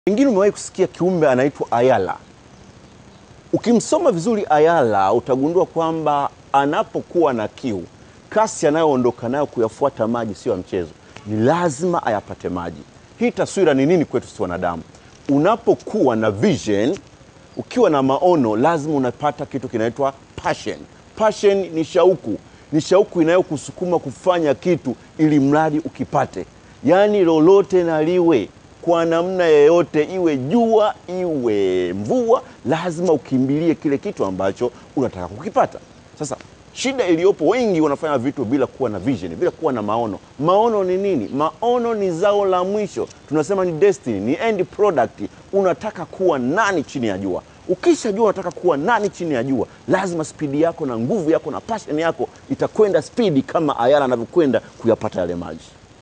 Hii ni Formula Namba Moja ya ku-Win!
Usipojua Unakwenda Wapi, utaishia popote!

Msemo huu ni moja ya misemo konki katika jamii. Kama hujui unakokwenda basi maana yake mtu yeyote anaweza kukubadilishia uelekeo.

Wapi unakwenda? Wapi ni destiny yako??? Au upo upo tu?

Kuijua destny yako kunategemea sana kama unalijua kusudi la kuumbwa kwako.

Sababu ya wewe kufinyangwa ni ipi?

Ukiijua sababu ya kuwepo, utaijua mbegu iliyopo ndani yako ambayo ndo itakusaidia kuzalisha matunda na kila utakapoamka utakuwa na shauku kama ayala kuyakimbilia maji.

Unasubiri nini? Ni DM nikuoneshe namna ya kukijua.
Kipaji.
Sababu kuumbwa...
Mbegu iliyomo ndani yako.